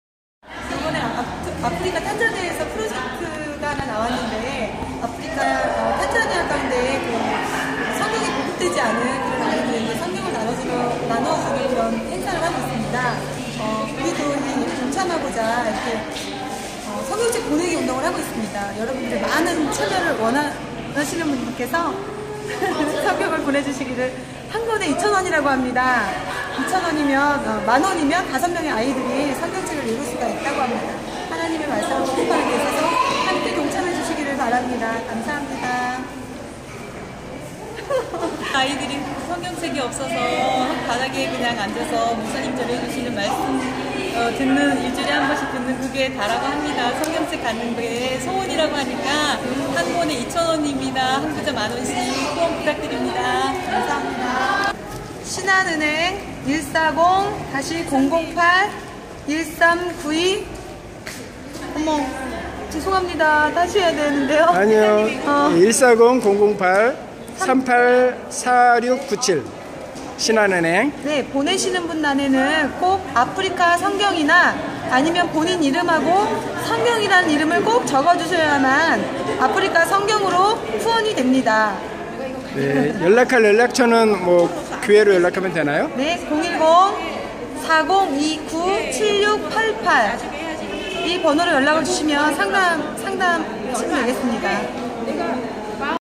Africa mission bible 이승아회장님과 회원들의 수고에 박수를 아프리카선교회 탄자니아 성경보급을 위해 후원을 요청하는 집사님들 인천순복음교회 해외선교위원회 예수그리스도
이승아회장님과 회원들의 수고에 박수를 아프리카선교회 탄자니아 성경보급을 위해 후원을 요청하는 집사님들 인천순복음교회 해외선교위원회 예수그리스도 살아계신 하나님의 아들